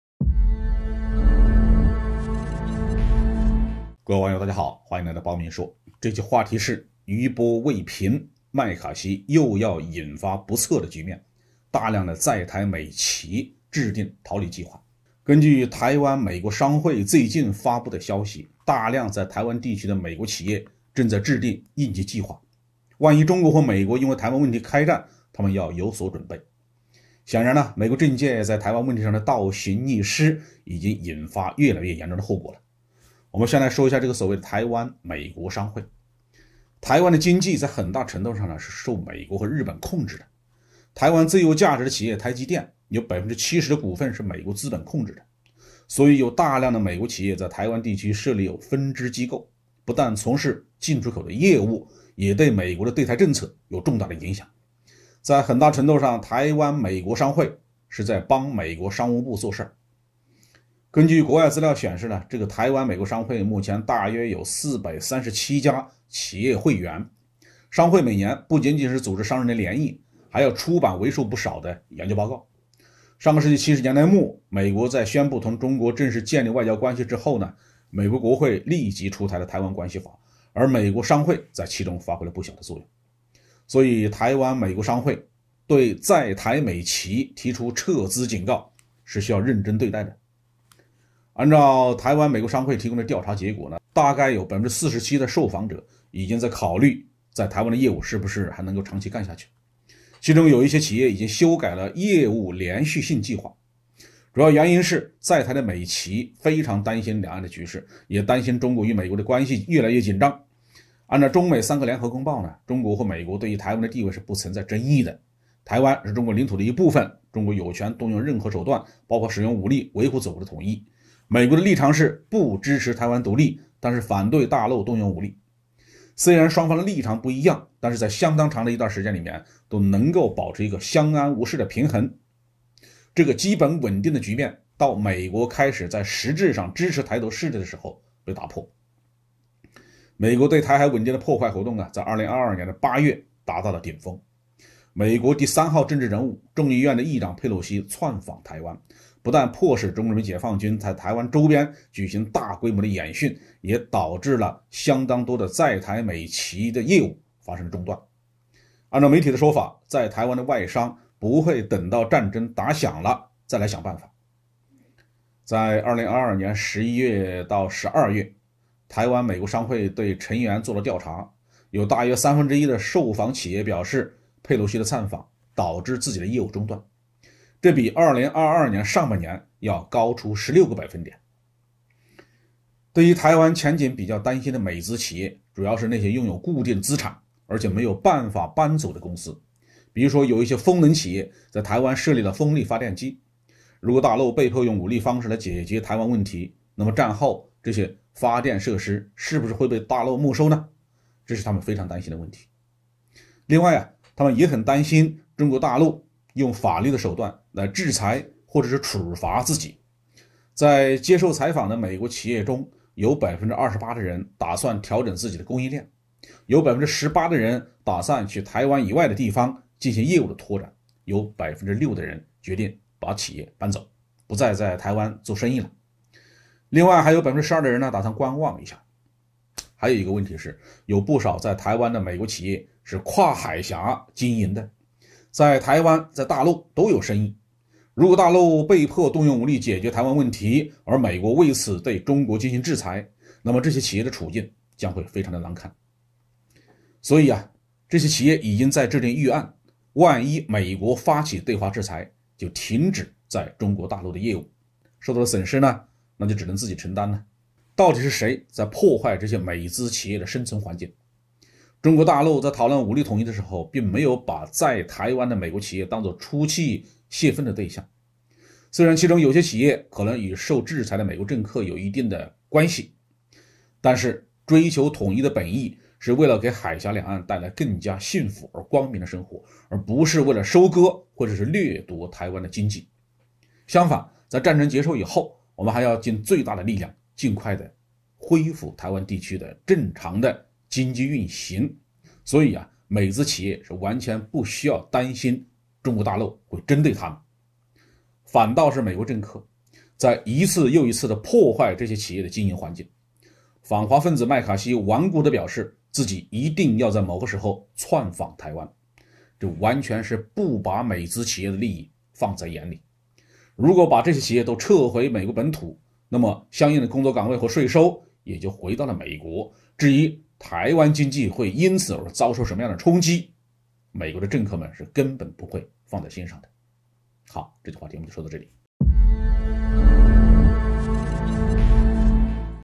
麥卡錫又要引發不測局面，大量在台美企制定逃離計劃，恐慌蔓延【包明大校】
餘波未平麥卡錫又要引發不測局面，大量在台美企制定逃離計劃
麥卡錫又要引發不測局面，大量在台美企制定逃離計劃，恐慌蔓延